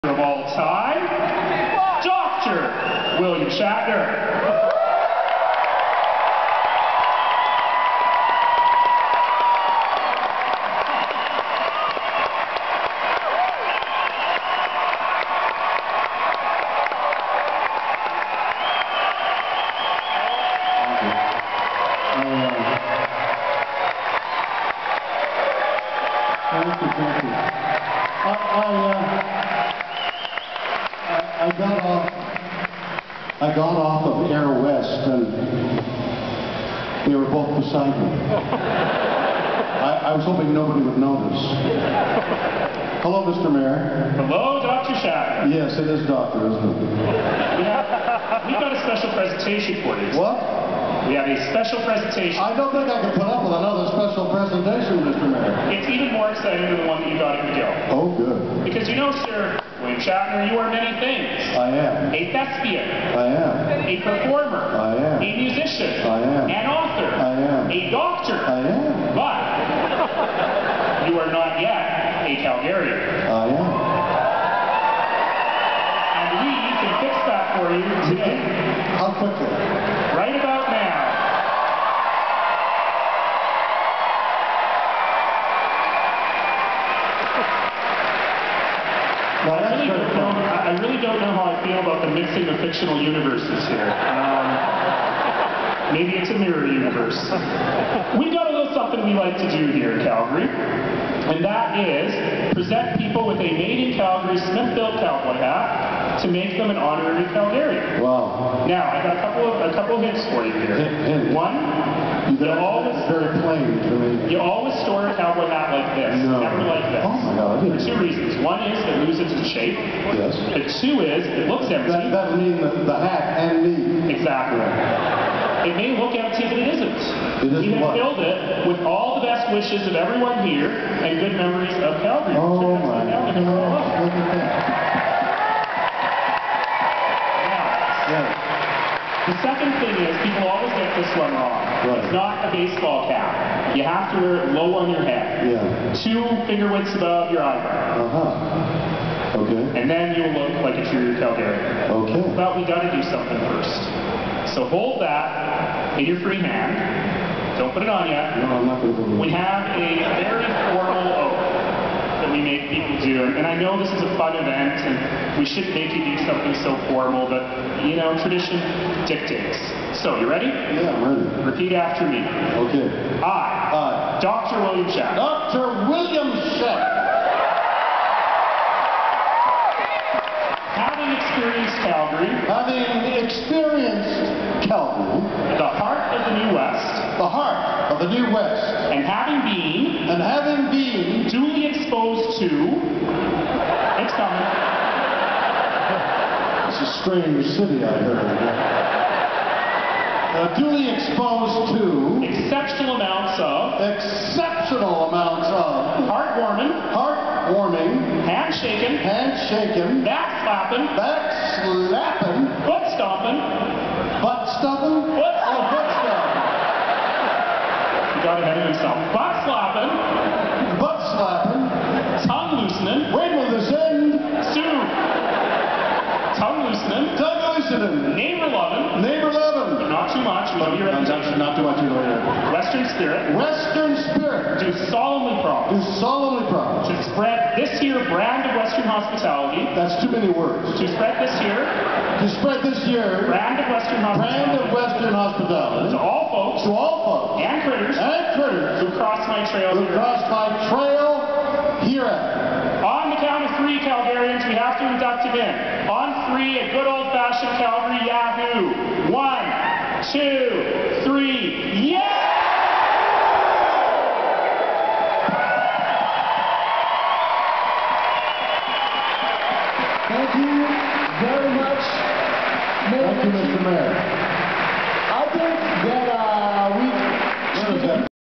William Shatner @ The Calgary Comic & Entertainment Expo 2011
A first few minutes of William Shatner's panel at the Calgary Comic & Entertainment Expo 2011.